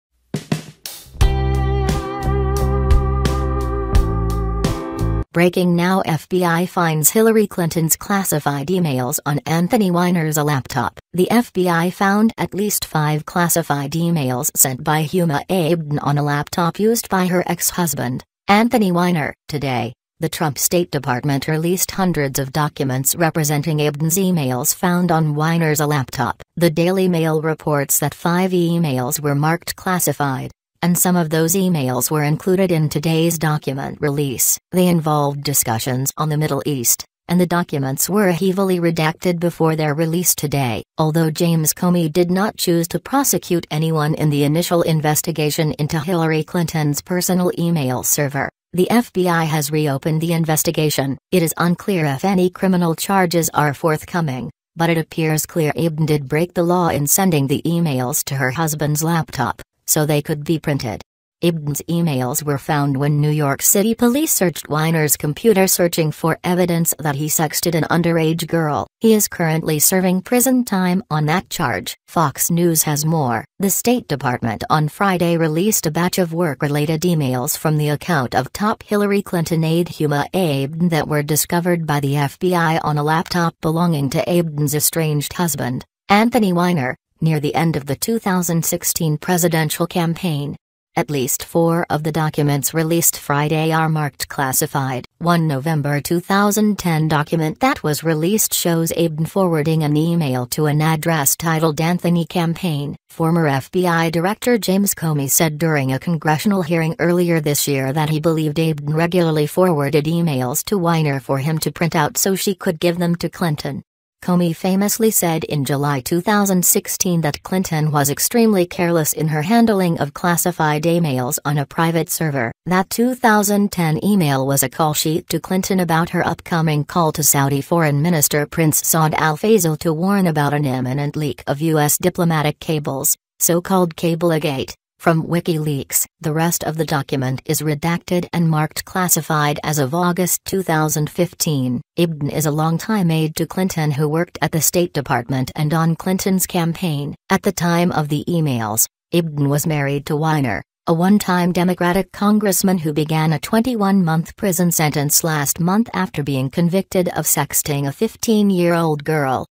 BREAKING NOW FBI FINDS HILLARY CLINTON’S CLASSIFIED EMAILS ON ANTHONY WEINER’S LAPTOP
Source Credit                                 silenceisconsent.net
The FBI found at least five classified emails sent by Huma Abedin on a laptop used by her ex-husband, Anthony Weiner.

Today, the Trump State Department released hundreds of documents representing Abedin’s emails found on Weiner’s laptop. The Daily Mail reports that five emails were marked classified, and some of those emails were included in today’s document release. They involved discussions on the Middle East, and the documents were heavily redacted before their release today.

Although James Comey did not choose to prosecute anyone in the initial investigation into Hillary Clinton’s personal email server, the FBI has reopened the investigation. It is unclear if any criminal charges are forthcoming, but it appears clear Abedin did break the law in sending the emails to her husband’s laptop, so they could be printed.

Abedin’s emails were found when New York City police searched Weiner’s computer searching for evidence that he sexted an underage girl. He is currently serving prison time on that charge.

Fox News has more.

The State Department on Friday released a batch of work-related emails from the account of top Hillary Clinton aide Huma Abedin that were discovered by the FBI on a laptop belonging to Abedin’s estranged husband, Anthony Weiner, near the end of the 2016 presidential campaign.

At least four of the documents released Friday are marked “classified.”

One November 2010 document that was released shows Abedin forwarding an email to an address titled “Anthony Campaign.”

Former FBI Director James Comey said during a congressional hearing earlier this year that he believed Abedin regularly forwarded emails to Weiner for him to print out so she could give them to Clinton.

Comey famously said in July 2016 that Clinton was “extremely careless” in her handling of classified emails on a private server.

That 2010 email was a “call sheet” to Clinton about her upcoming call to Saudi Foreign Minister Prince Saud al-Faisal to warn about an imminent leak of U.S. diplomatic cables — so-called Cablegate — from WikiLeaks.
The rest of the document is redacted and marked classified as of August 2015.

Abedin is a longtime aide to Clinton who worked at the State Department and on Clinton’s campaign.

At the time of the emails, Abedin was married to Weiner, a onetime Democratic congressman who began a 21-month prison sentence last month after being convicted of sexting a 15-year-old girl.